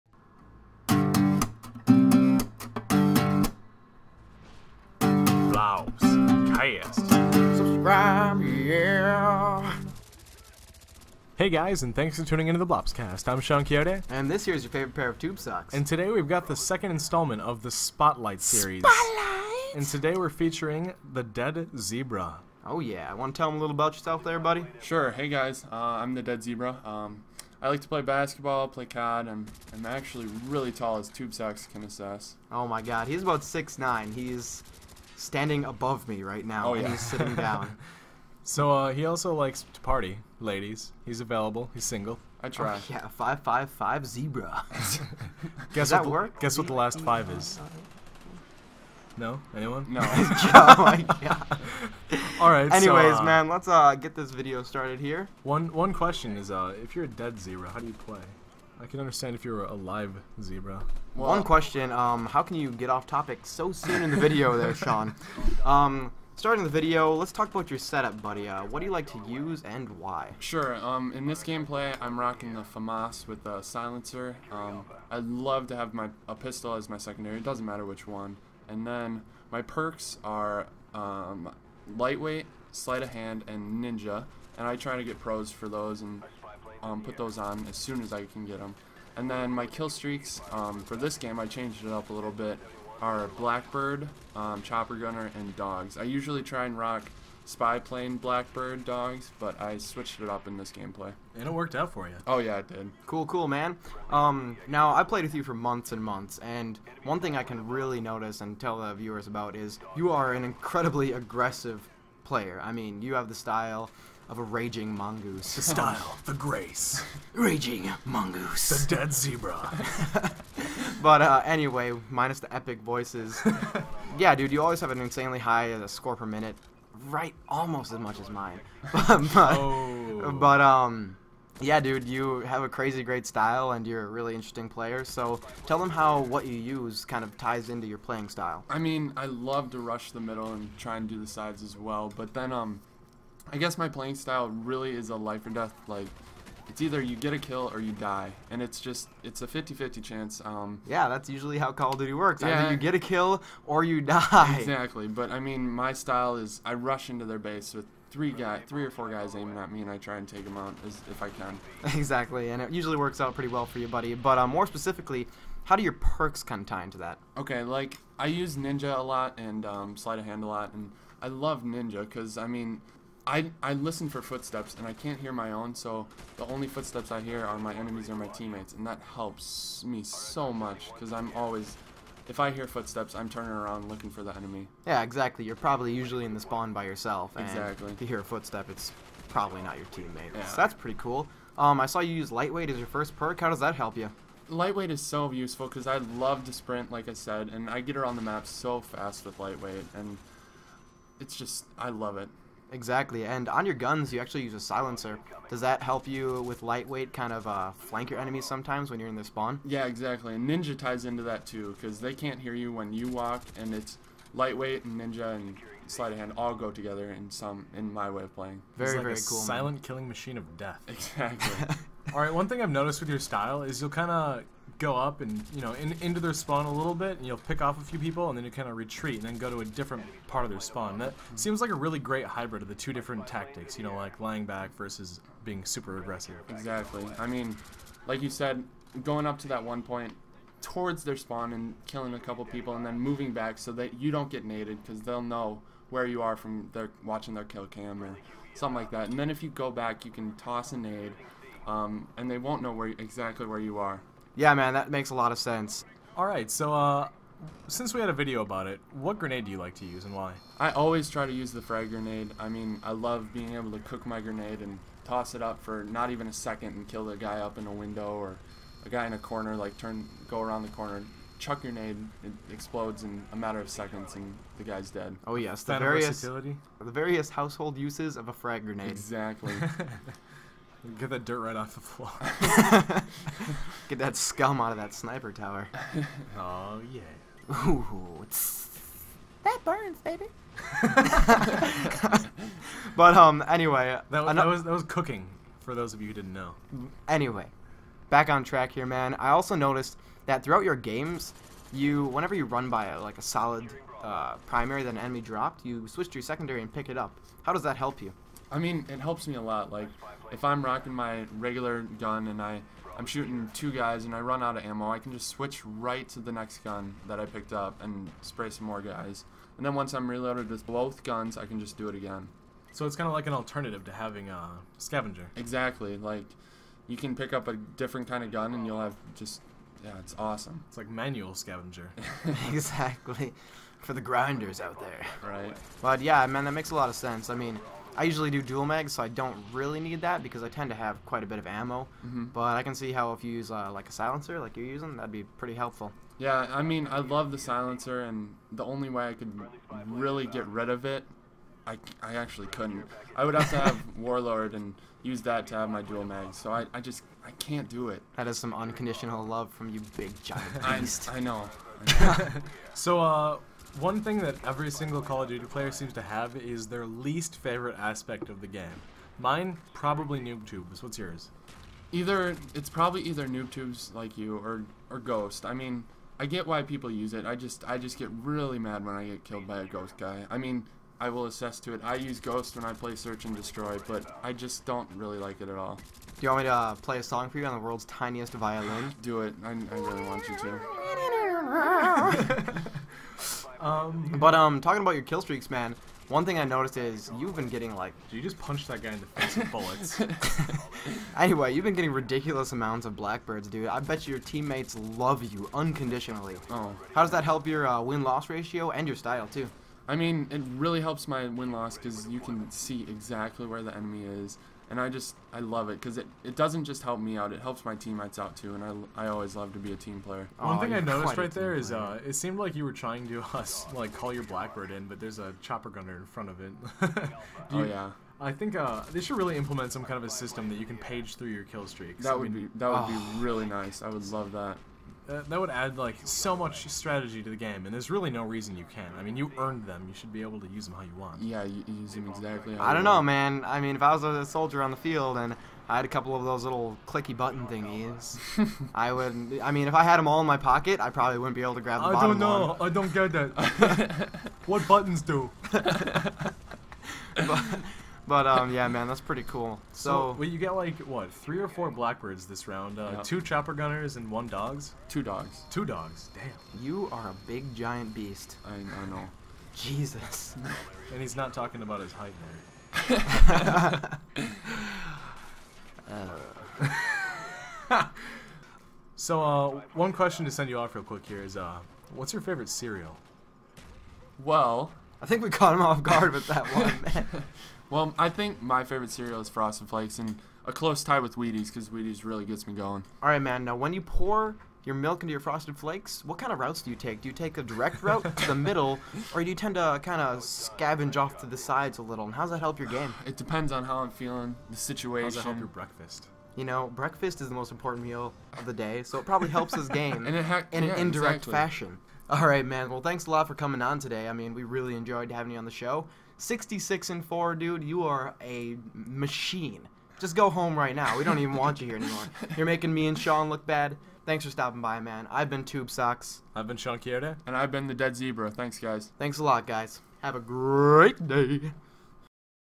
The Blopscast Spotlight: The Dead Zebra
Tubesox and Shawn Quixote delve into the mind and the skill of the talented and handsome "Dead Zebra."